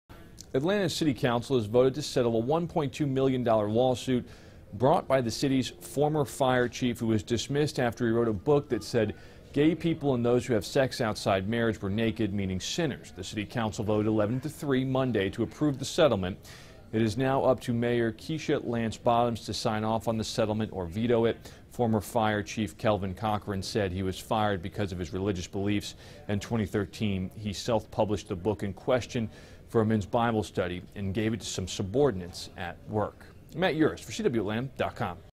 Atlanta City Council Votes To Settle Ex-Fire Chief's Lawsuit For $1.2 Million
Atlanta's City Council has voted to settle a $1.2 million lawsuit brought by the city's former fire chief who was dismissed after he wrote a book that included anti-gay passages. Matt Yurus reports.